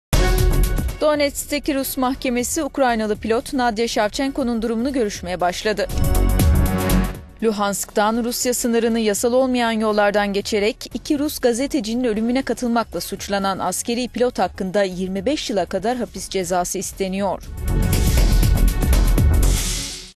Donetsk'teki Rus mahkemesi Ukraynalı pilotun durumunu görüşüyor
Donetsk'teki Rus mahkemesi Ukraynalı pilot Nadya Shavchenko'nun durumunu görüşmeye başladı.